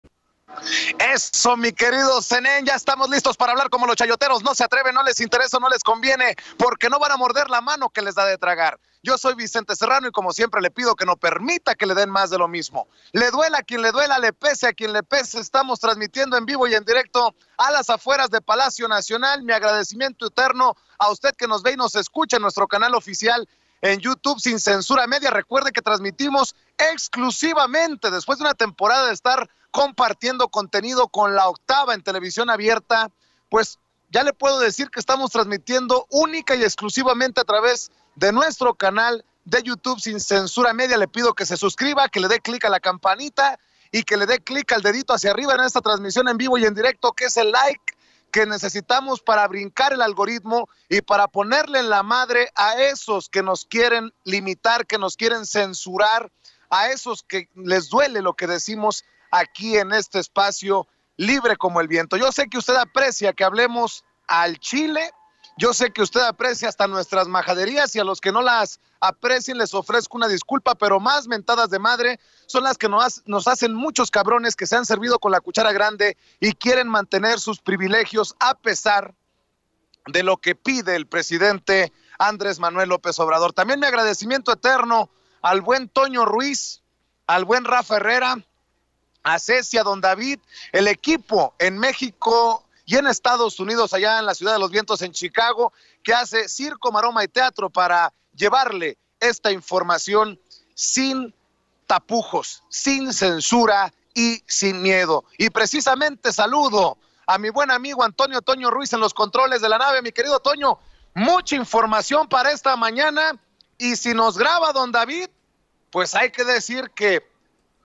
LLEGÓ VICENTE, PARA PONERLE EN LA MADRE A ESOS QUE NOS QUIEREN CENSURAR Y LIMITAR
Ya es hora de ponerle en la madre con noticias y con información, a esos que nos quieren censurar y limitar ¡Espíritu crítico!